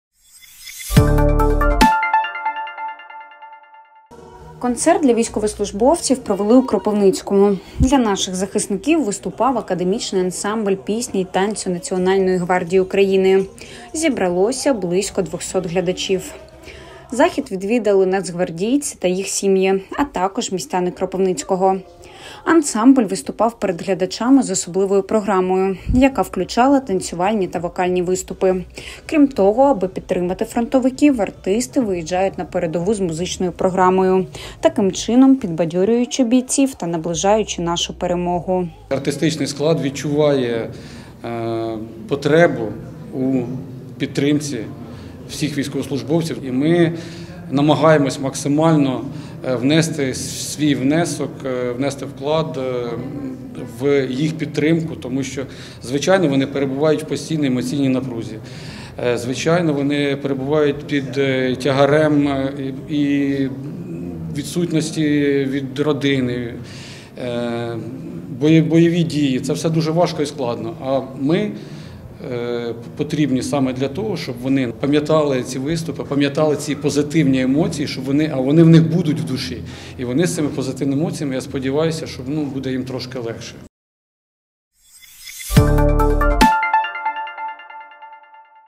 Концерт для військовослужбовців провели у Кропивницькому | CBN
Концерт для військовослужбовців провели у Кропивницькому. Для наших захисників виступав Академічний ансамбль пісні й танцю Національної гвардії України. Зібралося близько 200 глядачів. Захід відвідали нацгвардійці та їх сім'ї, а також містяни Кропивницького. Ансамбль виступив перед глядачами з особливою програмою, яка включала танцювальні та вокальні виступи. Крім того, аби підтримати фронтовиків, артисти виїжджають на передову з музичною програмою, яка підбадьорює бійців і таким чином наближаючи нашу перемогу.